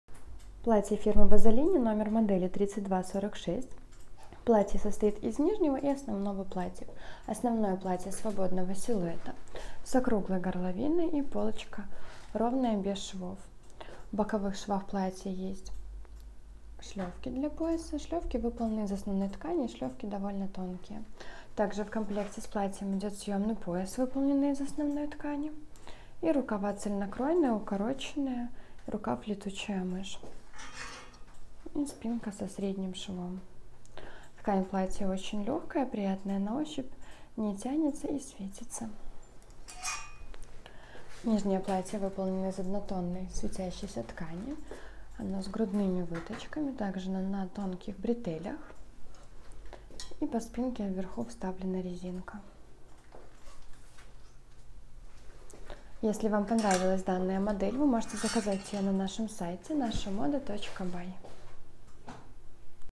Тип: Повседневные платья
Номер модели: 3246 .
фирмы : Bazalini.
Страна производства: Беларусь
Размеры: 48, 50, 52, 54, 56
Цвета: Как на фото
Рост: 170 см
Состав ткани: ПЭ 100% - шифон
Длина изделия, см: 104
Длина рукава, см: 30